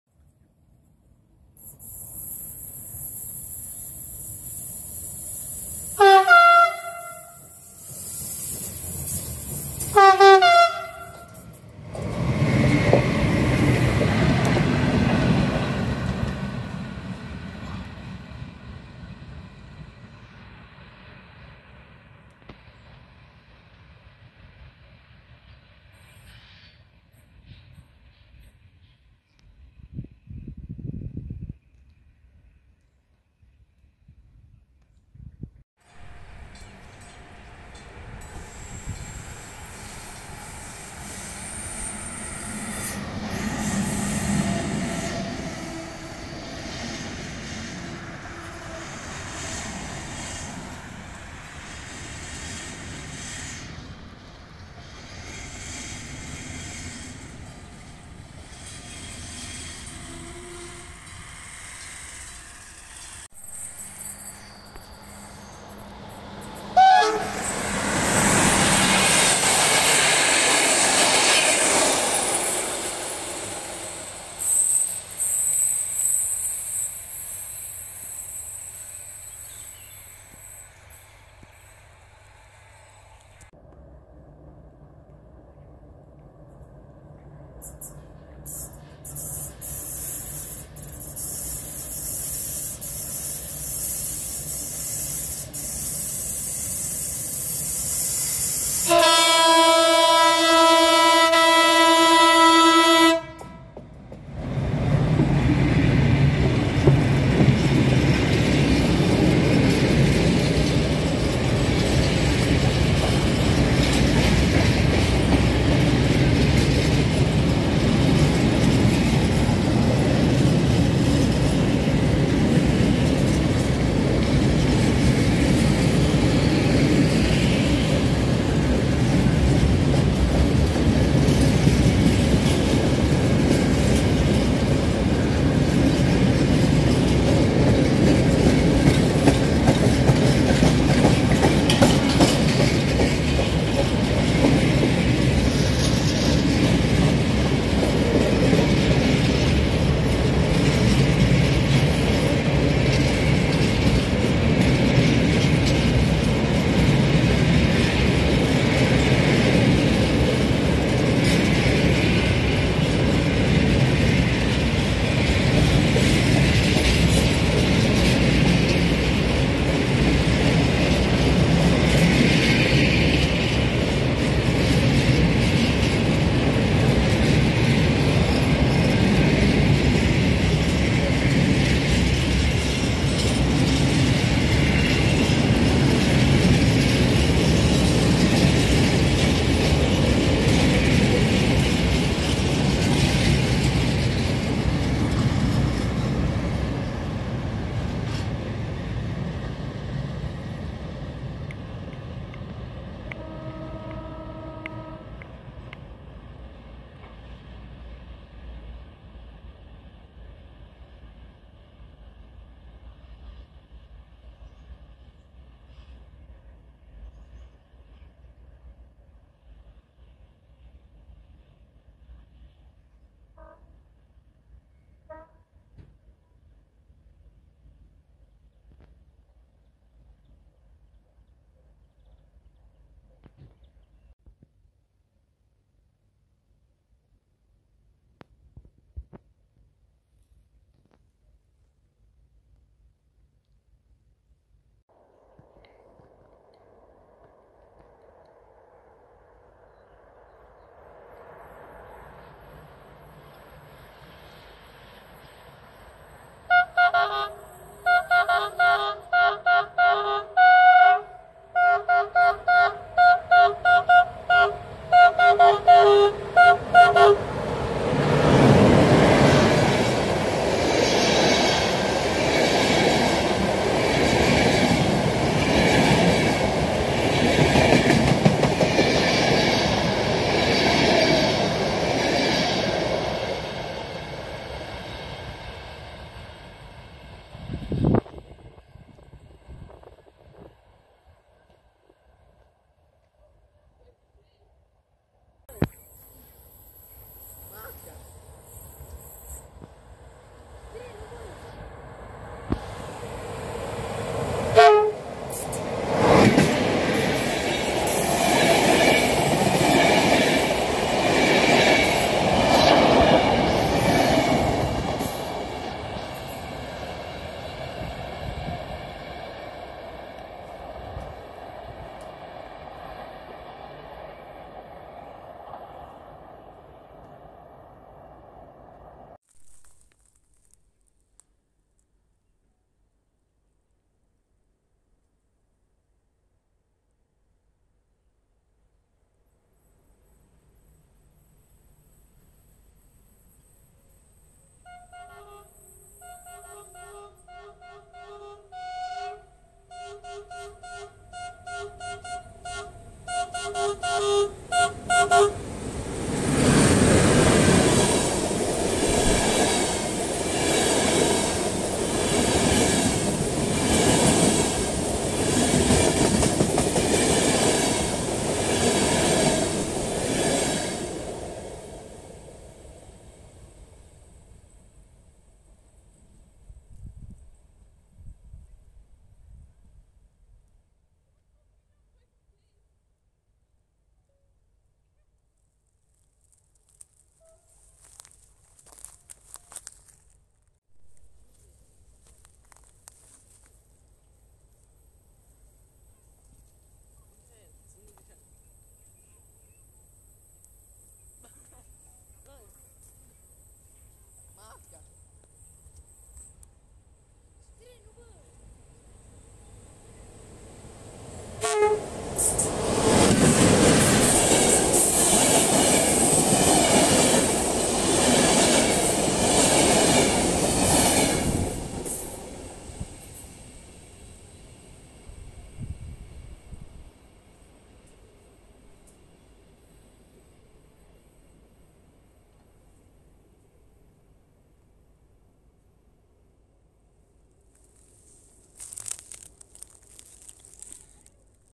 Trenuri de Paște în Drobeta Turnu Severin 🎉🎊🎋✨️🎀🎁
0:05 Izolate Tim Rail Cargo RO-TRC
0:40 IR 1691 București-nord to Timisoara-nord
1:05 Regio 9508 Drobeta-Turnu Severin to Craiova
1:25 Marfă CFR Marfă
4:10 IR 73-2 Budapest-keleti to București-nord
5:00 IR 349 București-nord to Arad
5:30 IR 73-2 Budapest-keleti to București-nord
6:35 IR 349 București-nord to Arad